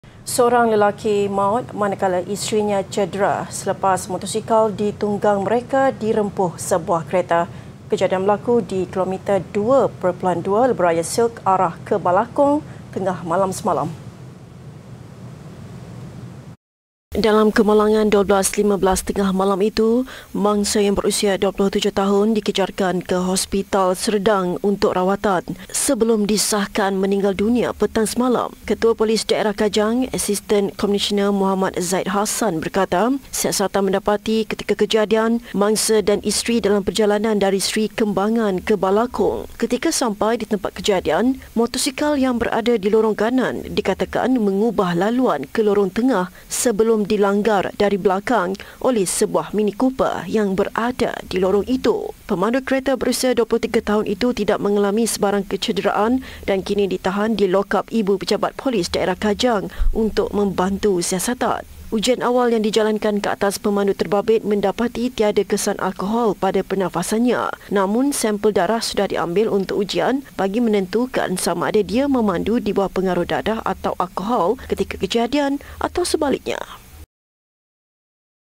Suami Maut Isteri Parah, Motosikal Dirempuh Kereta
Seorang lelaki maut manakala isterinya cedera selepas motosikal ditunggang mereka dirempuh sebuah kereta di Kilometer 2.2 Lebuhraya SILK arah ke Balakong tengah malam semalam.